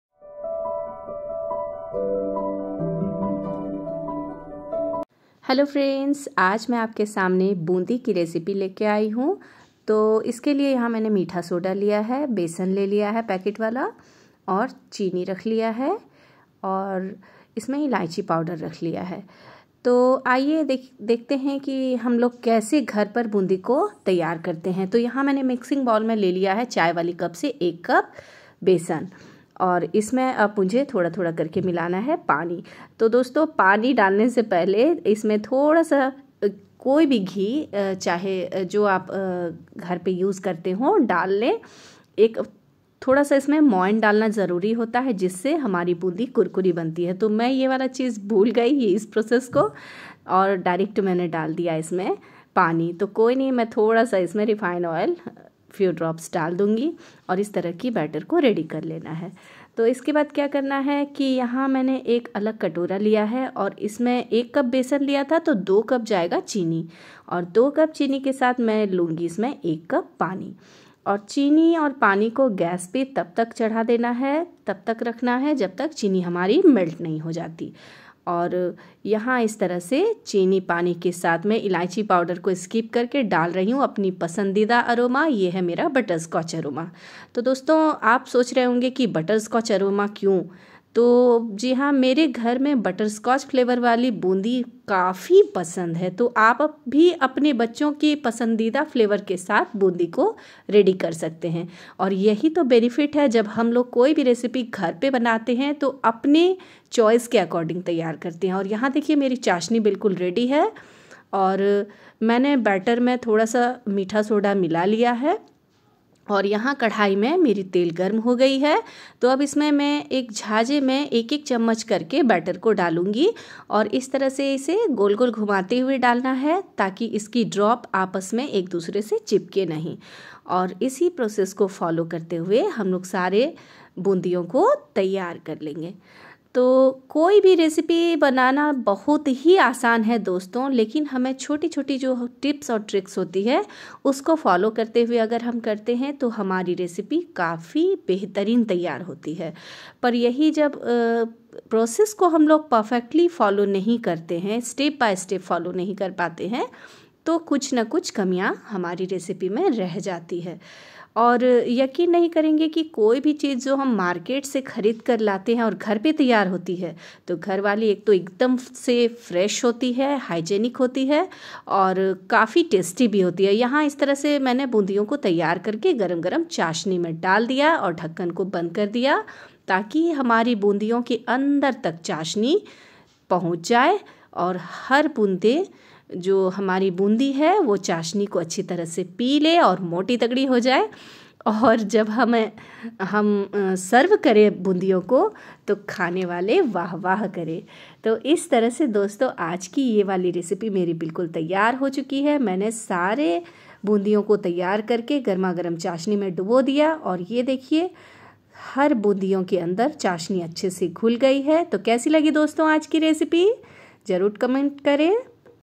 प्रसाद वाली बूंदी बनाएं मेरे तरीके से | बेसन की बूंदी | Dreampuff Homebaker
ingredients for boondi

butterscotch aroma 1 tbs
baking soda 1 pinch
refined oil approx 150g